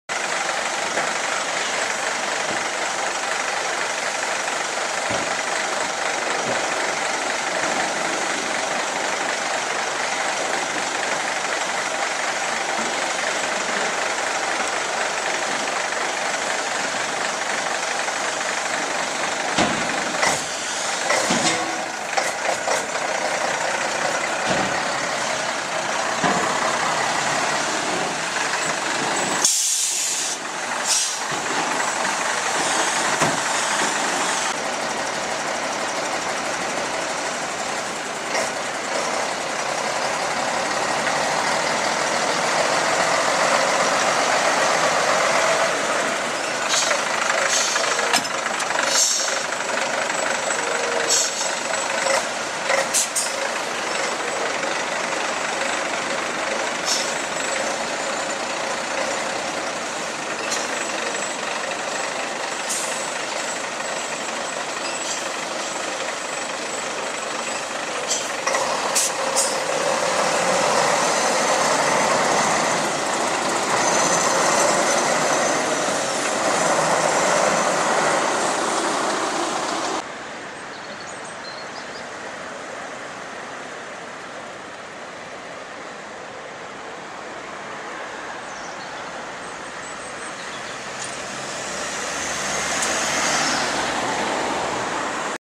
Dodge-Barreiros C20
Un precioso Dodge maniobrando en el ferial de ganado de Torrelavega este miercoles, luego lo vemos marchar por las calles de Torrelavega, y es que pense que tardaria mas en salir y me hizo correr para cogerle.